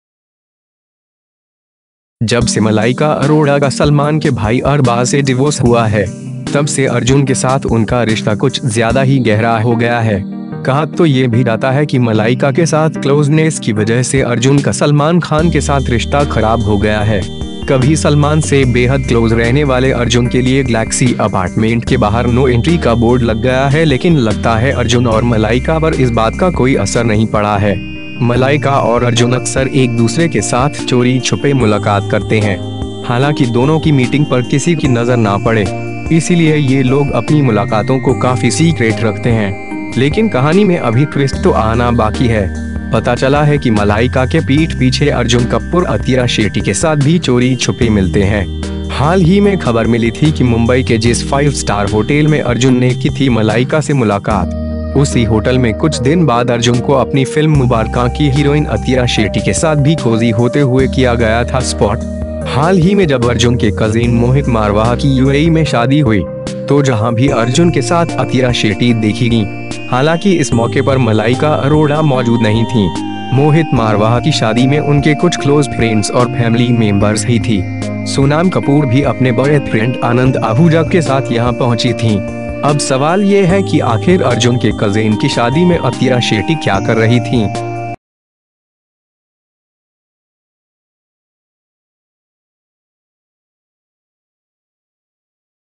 क्या अर्जुन कपूर एक साथ दो हसीनाओं को डेट कर रहे हैं
जब से मलाइका अरोड़ा का सलमान के भाई अरबाज़ से डिवोर्स हुआ है.. तब से अर्जुन के साथ उनका रिश्ता कुछ ज़्यादा ही गहरा हो गया है.. कहा तो ये भी जाता है कि मलाइका के साथ क्लोज़नेस की वजह से अर्जुन का सलमान ख़ान के साथ रिश्ता ख़राब हो गया है। कभी सलमान से बेहद क्लोज़ रहने वाले अर्जुन के लिए ग्लैक्सी अपार्टमेन्ट के बाहर नो इंट्री का बोर्ड लग गया है लेकिन लगता है अर्जुन और मलाइका पर इस बात का कोई असर नहीं पड़ा है. मलाइका और अर्जुन अक्सर एक दूसरे के साथ चोरी छुपे मुलाक़ात करते हैं। हालांकि दोनों की मीटिंग पर किसी की नज़र ना पड़े. इसीलिए ये लोग अपनी मुलाक़ातों को काफी सीक्रेट रखते हैं।

लेकिन कहानी में अभी ट्विस्ट तो आना बाक़ी है। पता चला है कि मलाइका के पीठ पीछे अर्जुन कपूर अतिया शेट्टी के साथ भी चोरी छुपे मिलते हैं। हाल ही में ख़बर मिली थी कि मुंबई के जिस फाइव स्टार होटल में अर्जुन ने की थी मलाइका से मुलाक़ात.. उसी होटल में कुछ दिन बाद अर्जुन को अपनी फिल्म मुबारकां की हीरोइन अतिया शेट्टी के साथ भी कोज़ी होते हुए किया गया था स्पॉट । हाल ही में जब अर्जुन के कज़ेन मोहित मारवाह की यूएई में शादी हुई..
तो जहां भी अर्जुन के साथ अतिया शेट्टी देखी गईं। हालांकि इस मौक़े पर मलाइका अरोड़ा मौजूद नहीं थीं... मोहित मारवाह की शादी में उनके कुछ क्लोज़ फ्रेन्ड्स और फैमली मेम्बर्स ही थी.. सोनाम कपूर भी अपने बॉयफ्रेन्ड आनंद आहूजा के साथ यहां पहुंची थीं...
अब सवाल ये है कि आख़िर अर्जुन के कज़ेन की शादी में अतिया शेट्टी क्या कर रही थीं